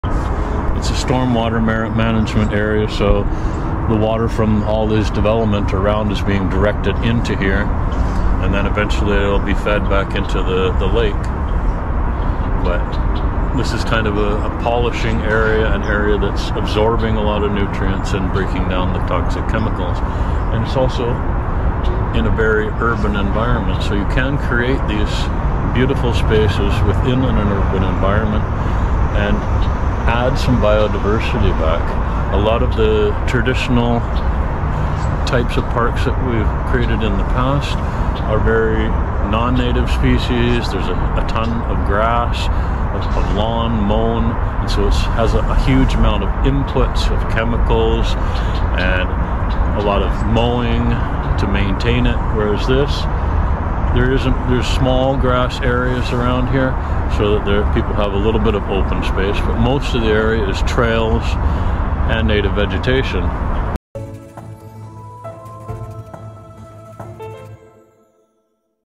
Transforming Urban Spaces: The Power of Stormwater Management
Discover how stormwater management areas can beautify urban environments while enhancing biodiversity. We explore innovative approaches to absorb nutrients and mitigate toxins, promoting a healthier ecosystem for city dwellers. Join us in redefining green spaces!